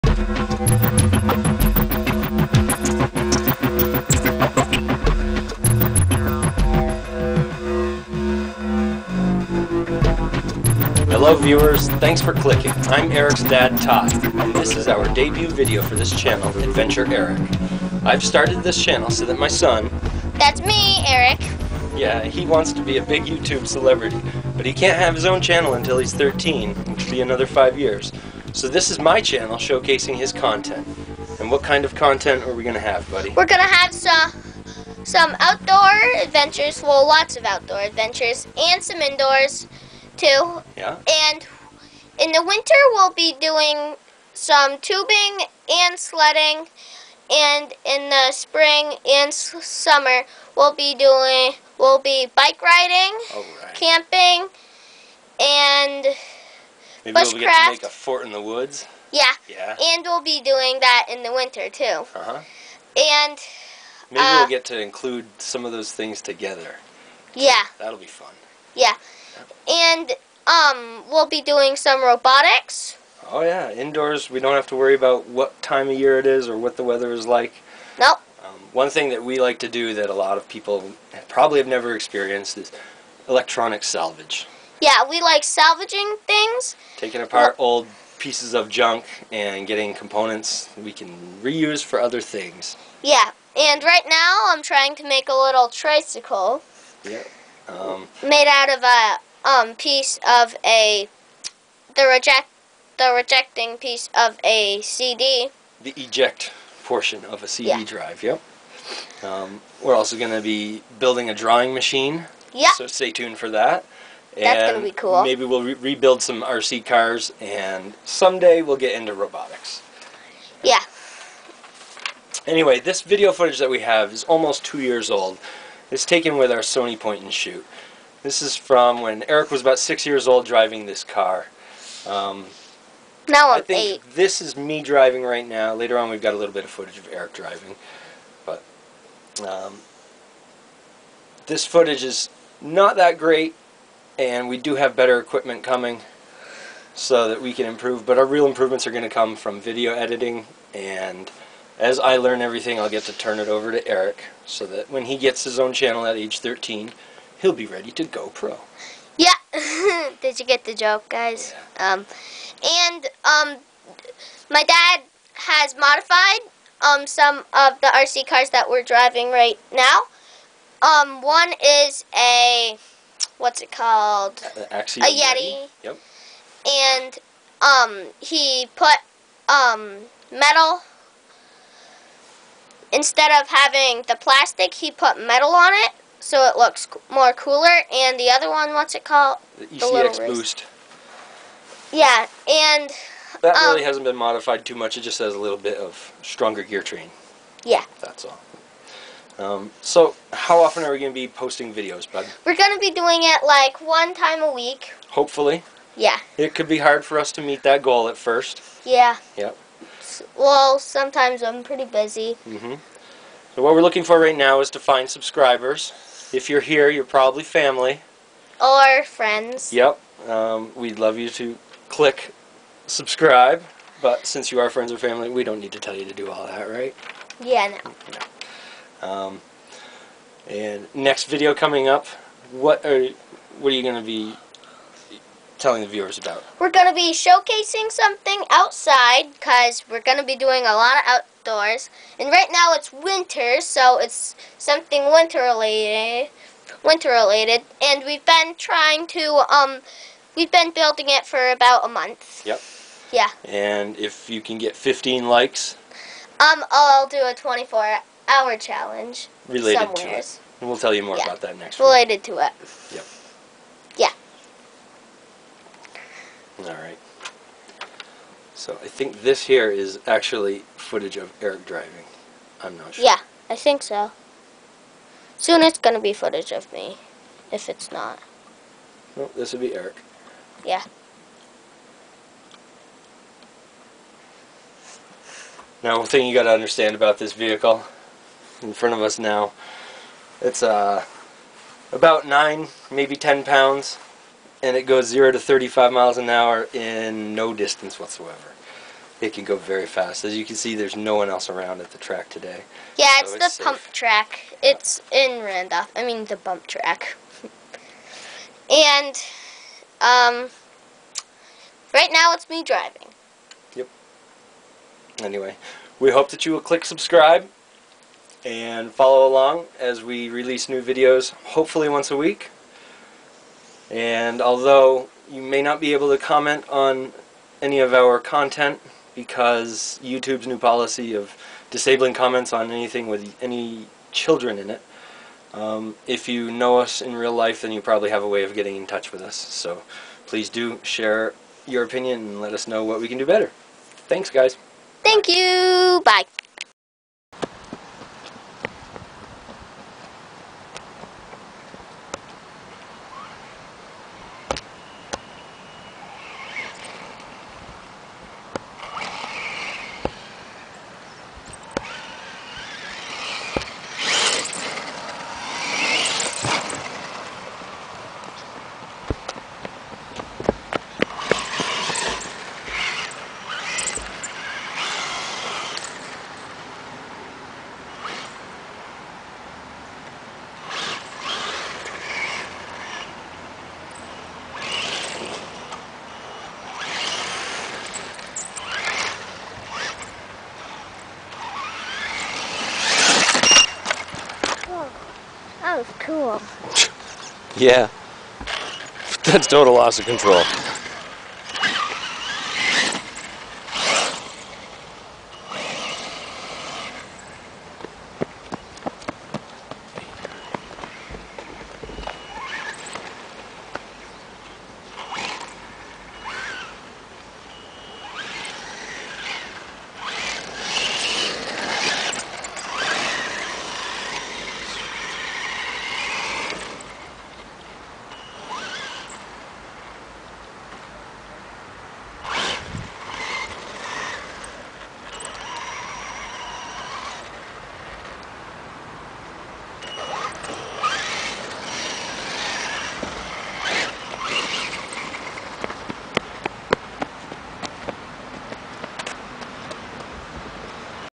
AdventureAeryck Channel Debut
Debut Video for AdventureAeryck featuring old footage of RC cars at the track.  Aeryck and Dad give the gist of what the channel will bring, and we beg and plead for your subscription.